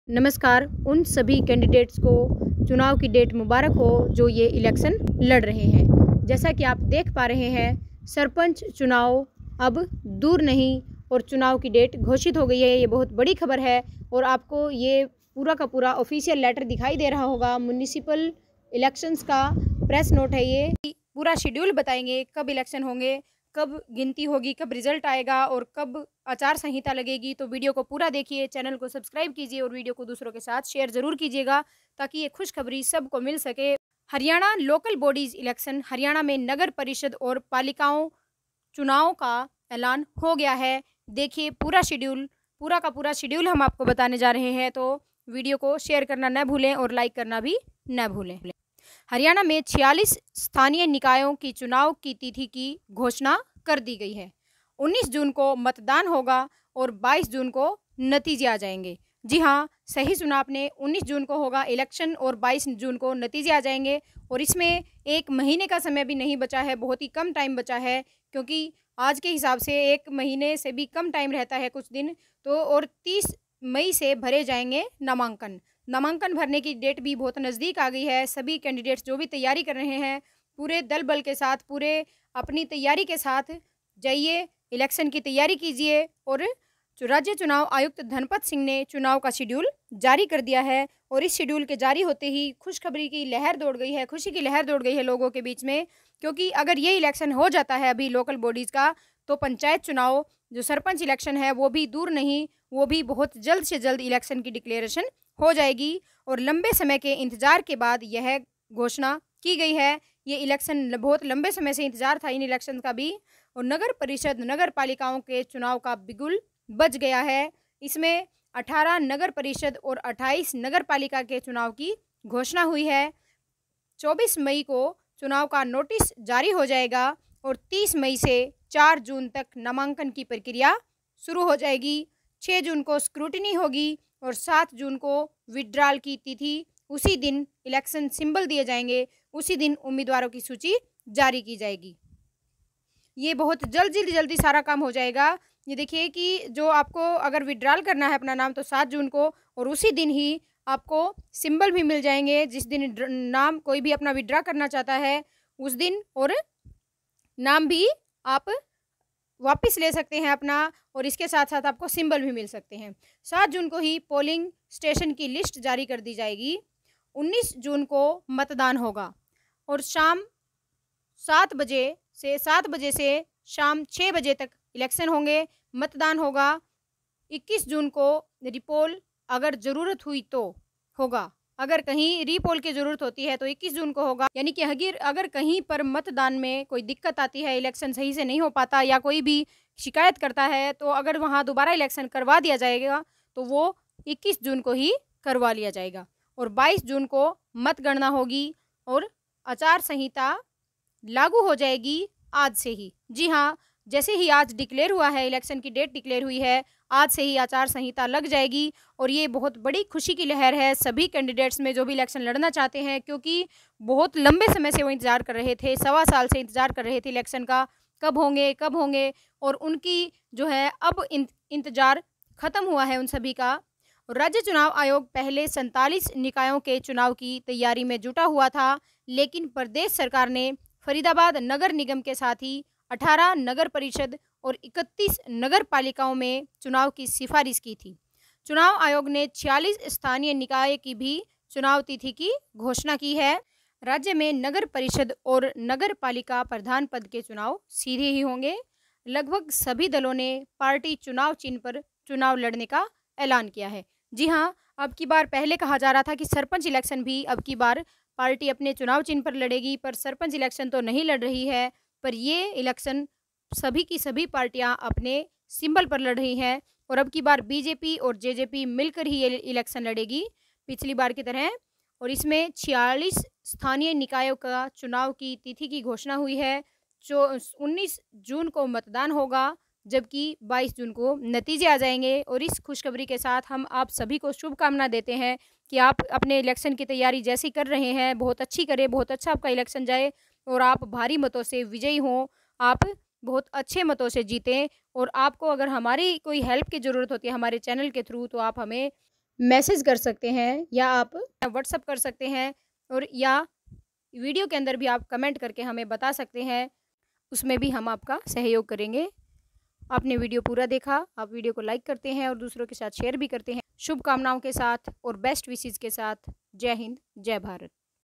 सरपंच चुनाव अब दूर नहीं चुनाव का ऐलान चंडीगढ़ से लाइव
17 मई को नगर निकाय का कोर्ट का क्या फैसला रहा